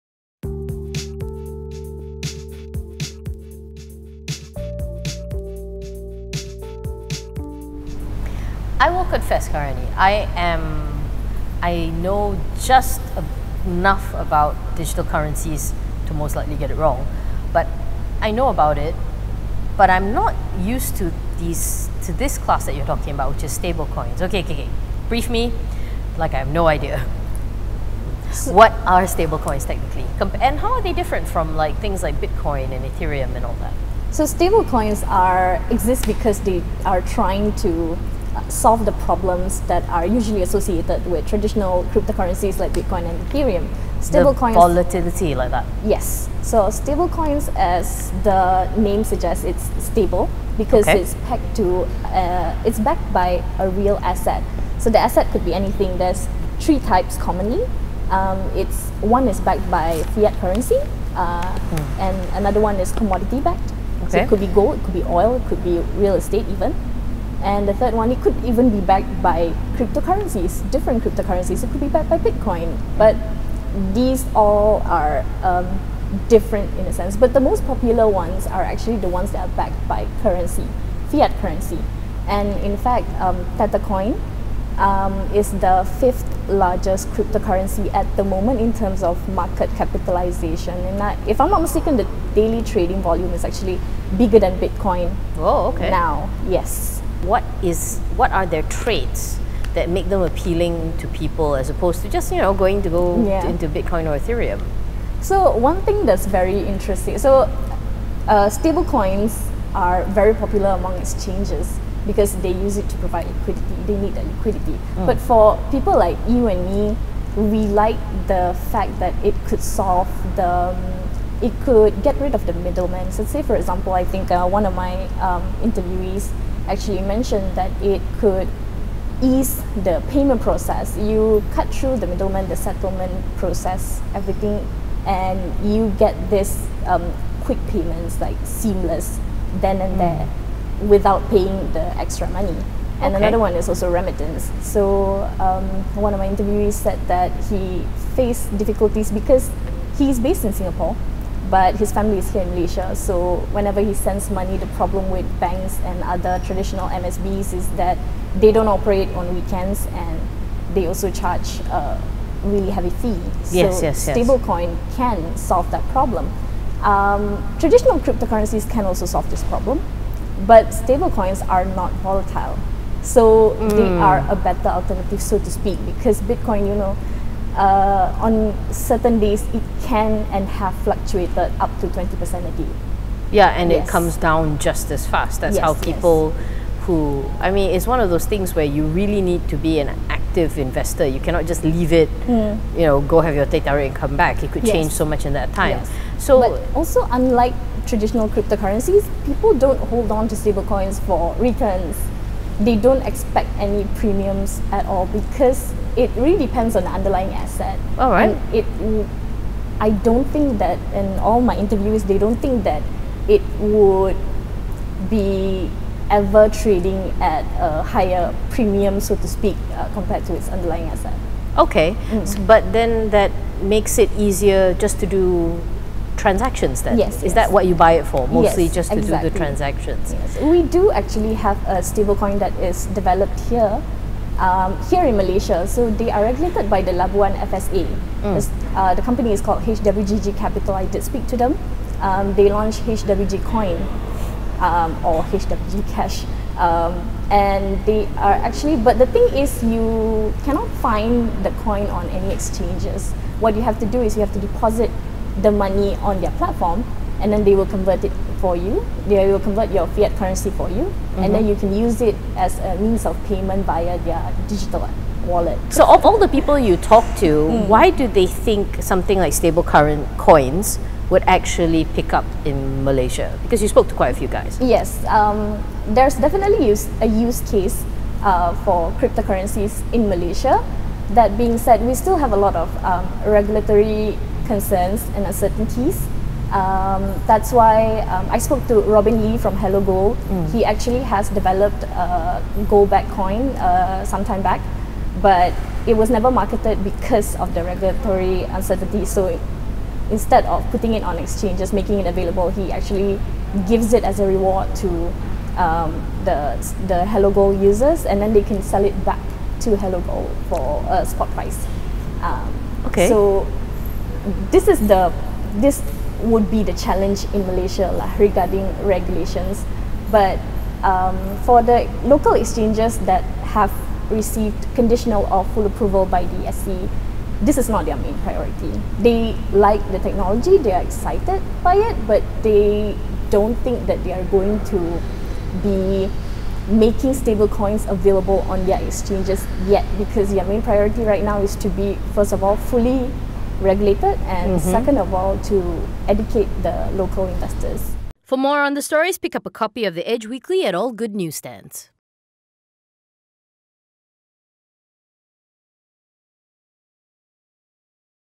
BEHIND THE STORY: Stablecoins on a growth trajectory
Bitcoin and ethereum are better known examples of digital currency, so what exactly are stablecoins and where do they fit in the ecosystem?